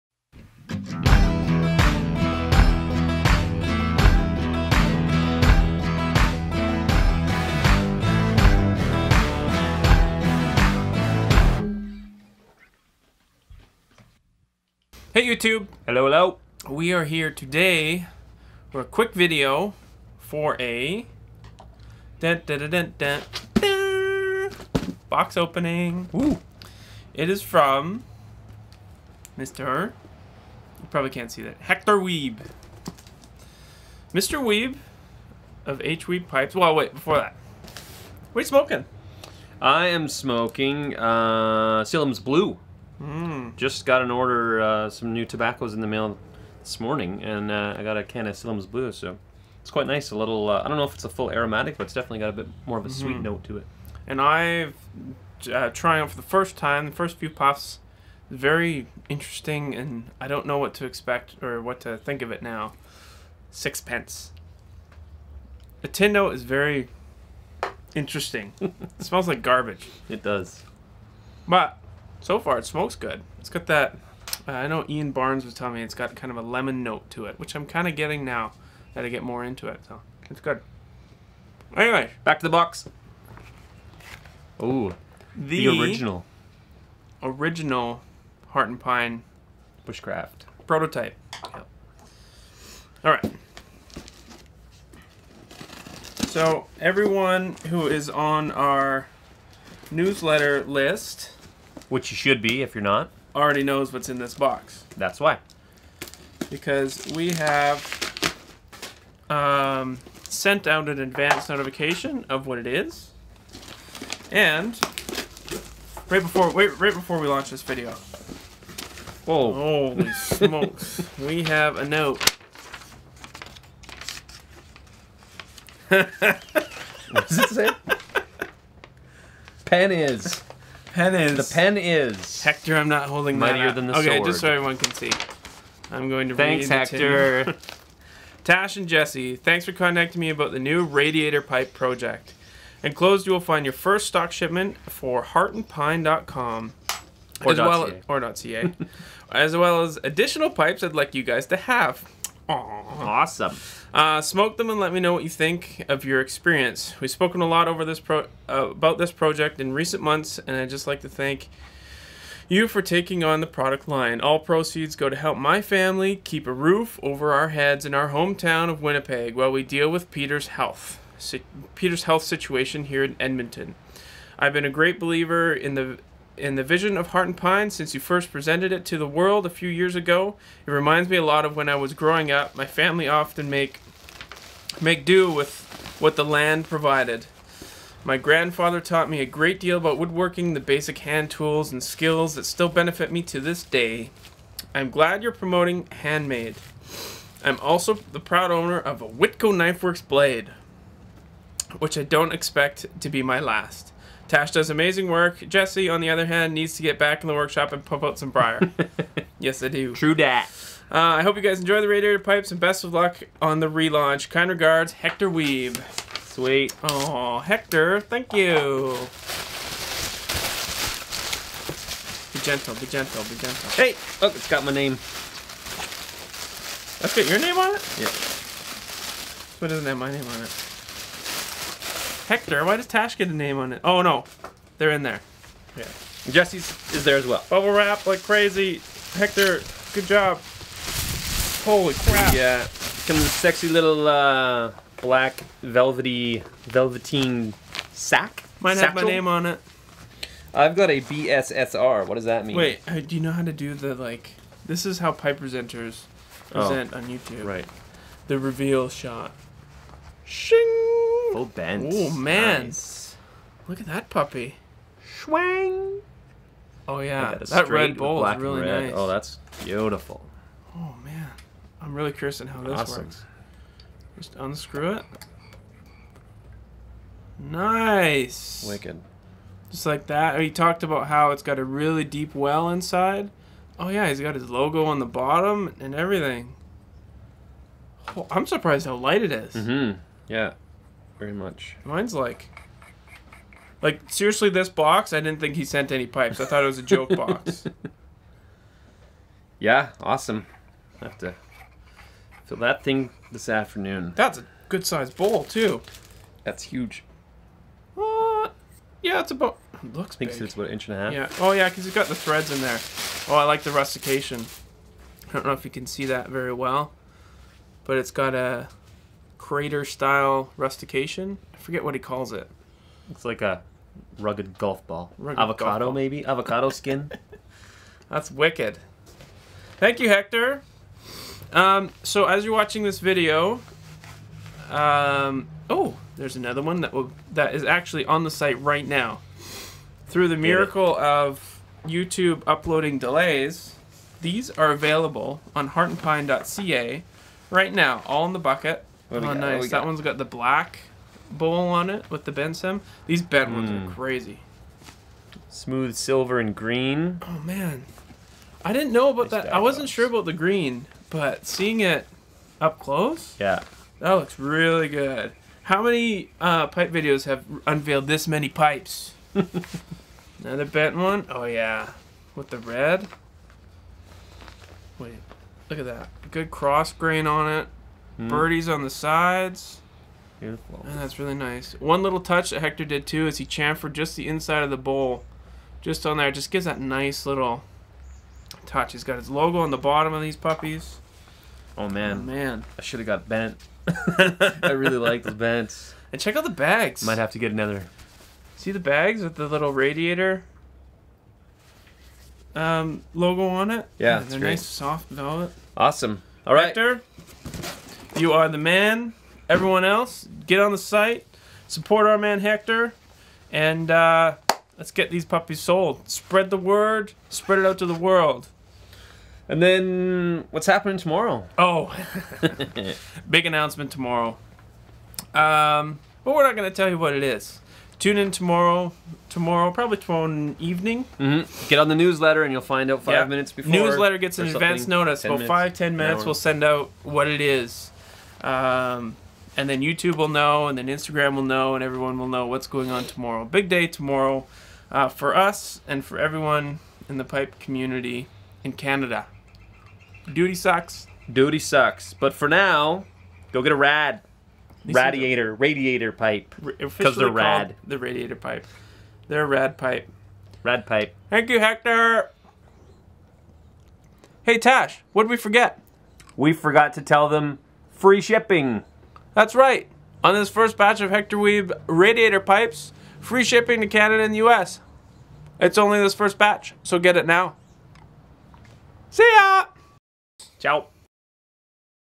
Hart and Pine - Announcing H. Wiebe Radiator Pipes!
Our first box opening from our pal Hekthor Wiebe. The perfect way to announce that we now have the infamous radiator pipe available!

Hekthor on IG - @hwiebepipes